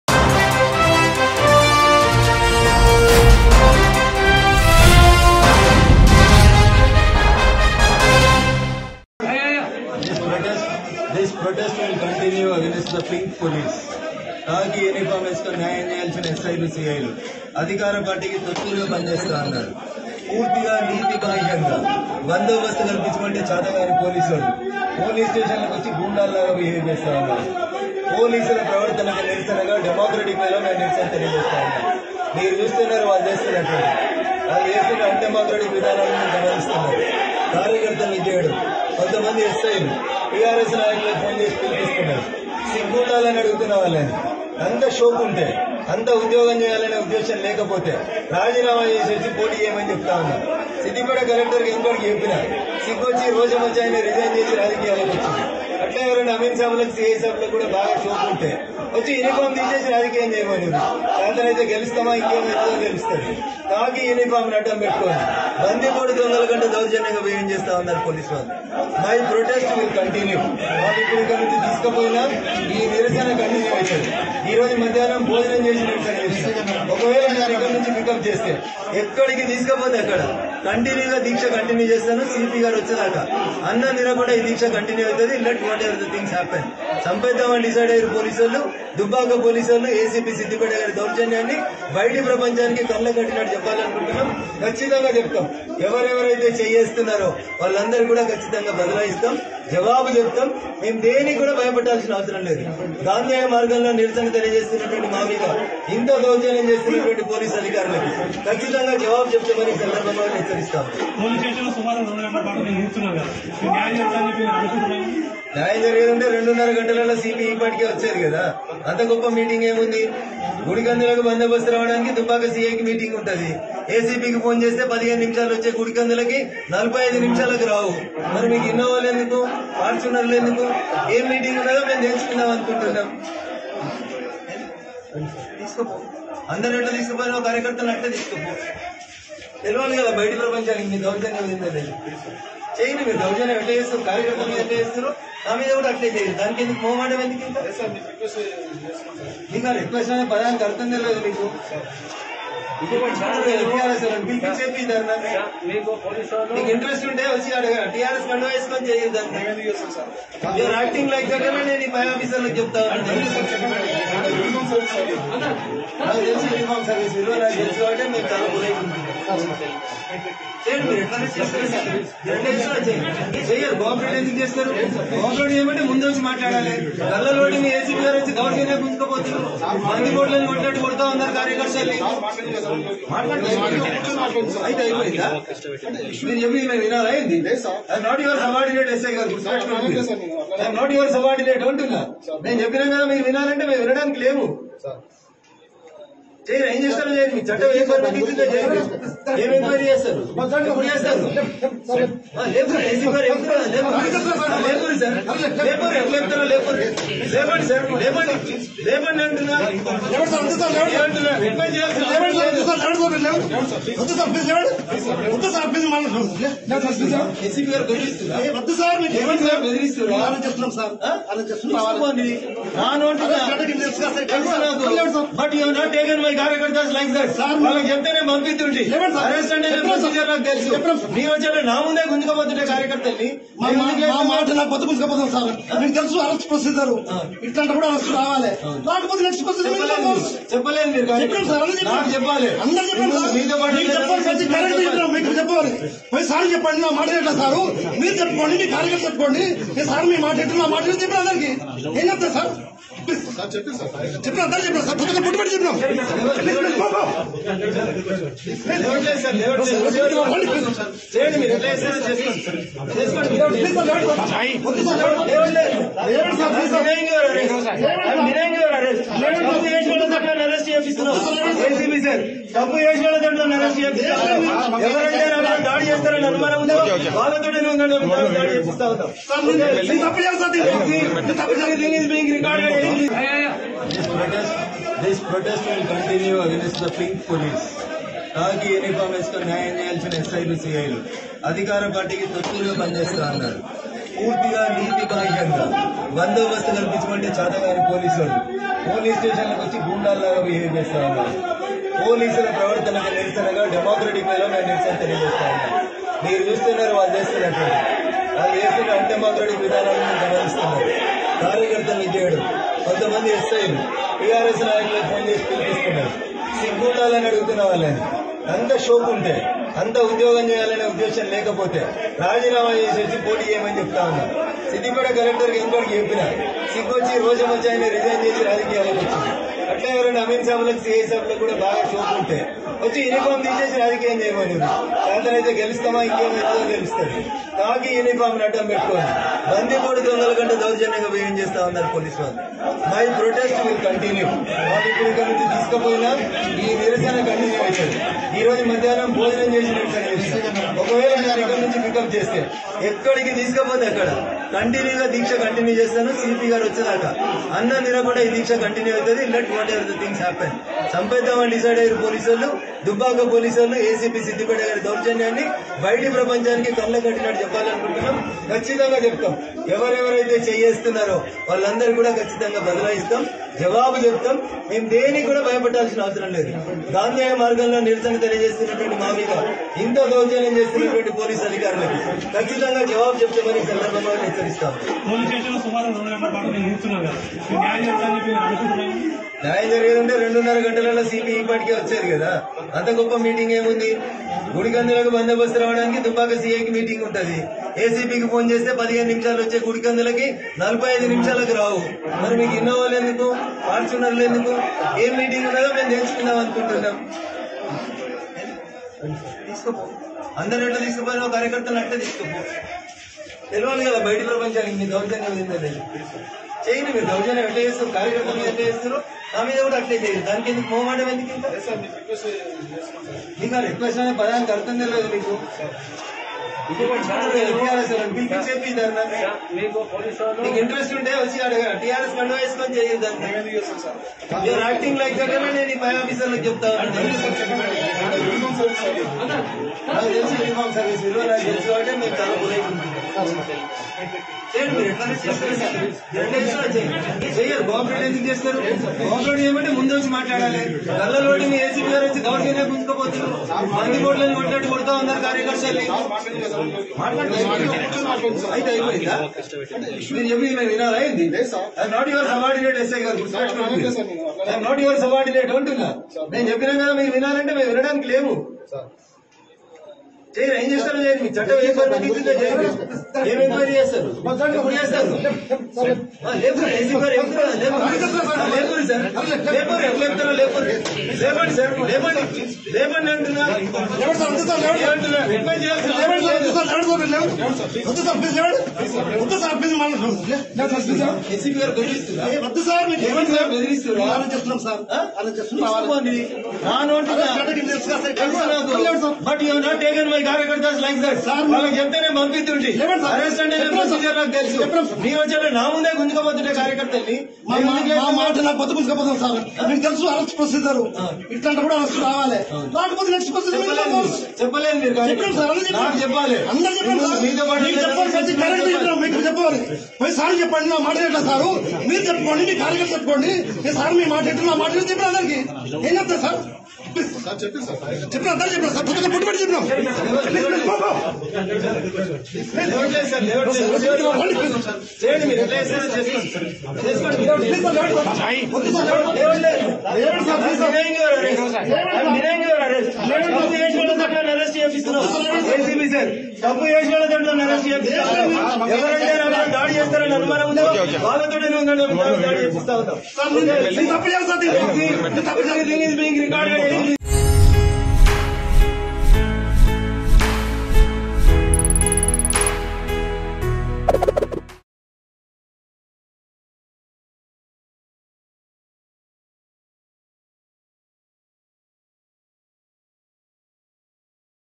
Dubbaka BJP MLA Raghunandan Rao Challenges Telangana Police In Station | Telangana News | Mango News
Dubbaka BJP MLA Raghunandan Rao Challenges Telangana Police In Station.